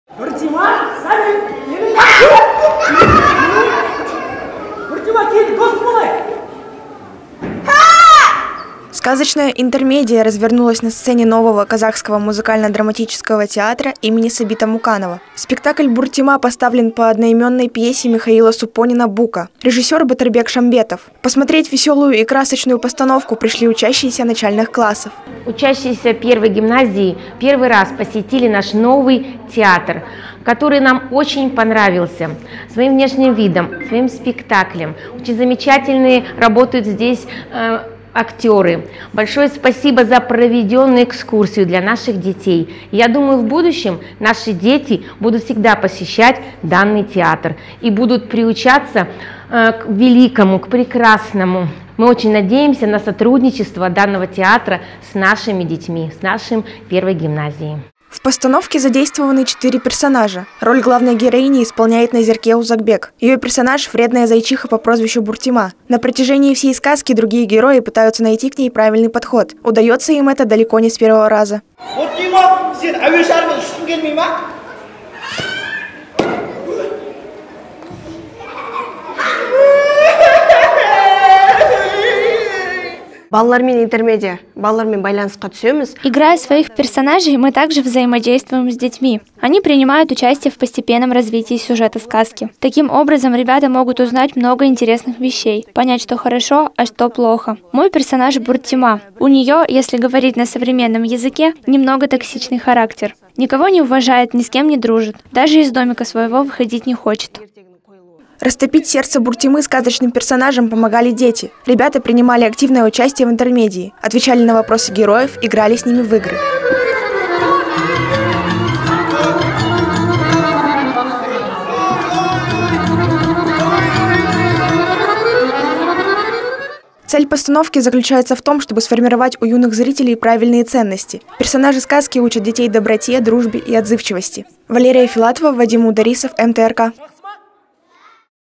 Спектакль "Бұртима" показали юным североказахстанцам
Сказочная интермедия развернулась на сцене нового казахского музыкально-драматического театра им. С. Муканова. Спектакль "Бұртима" поставлен по одноименной пьесе Михаила Супонина "Бука". Режиссёр - Батырбек Шамбетов. Посмотреть весёлую и красочную постановку пришли учащиеся начальных классов.